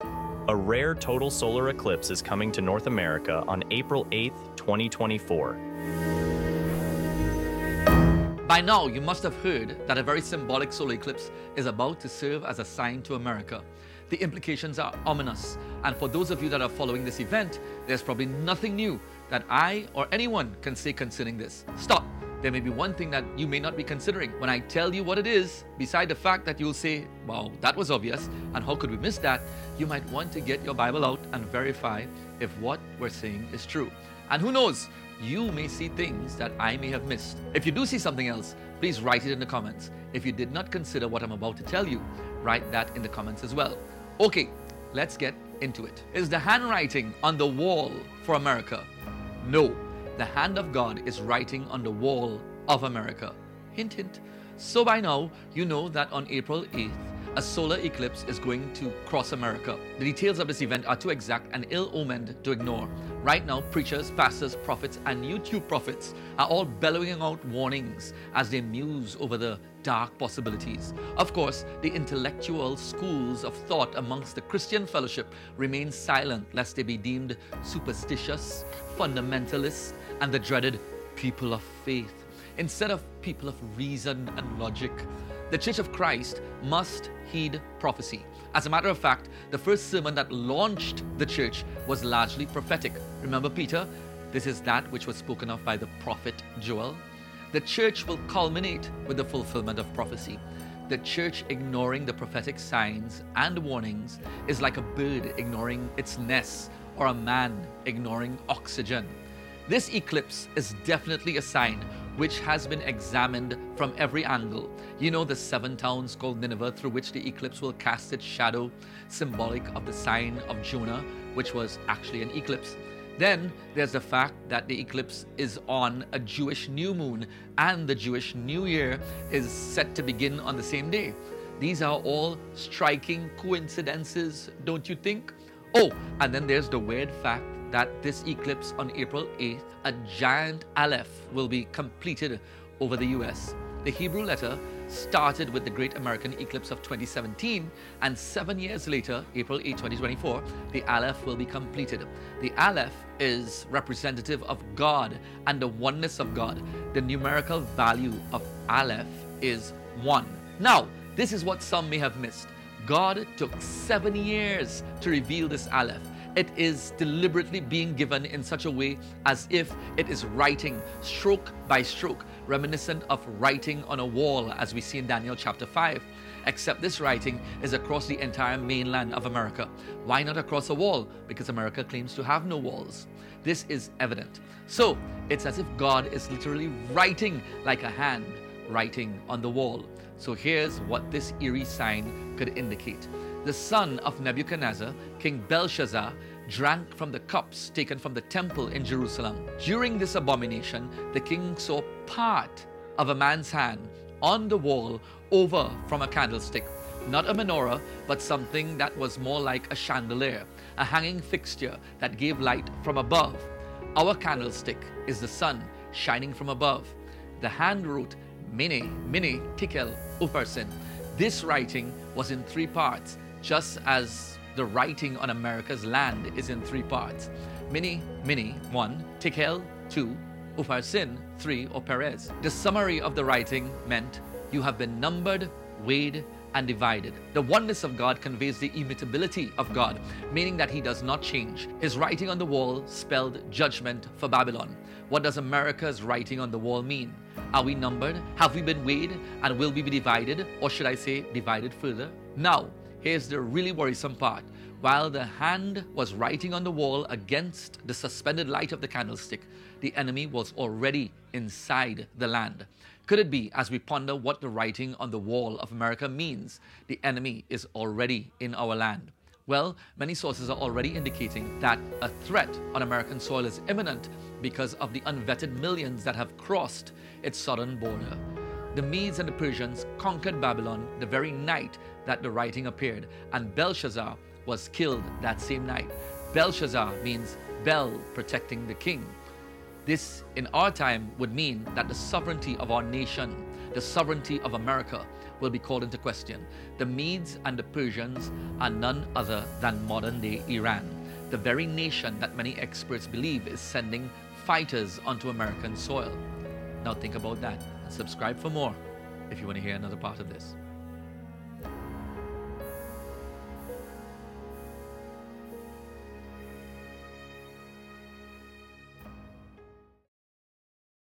Solar Eclipse April 8th 2024
What Is The Upcoming Solar Eclipse Really Telling Us? IS this NOT reminiscent of Daniel Chapter 5 where the hand of a man was writing on the wall. What does this mean for America?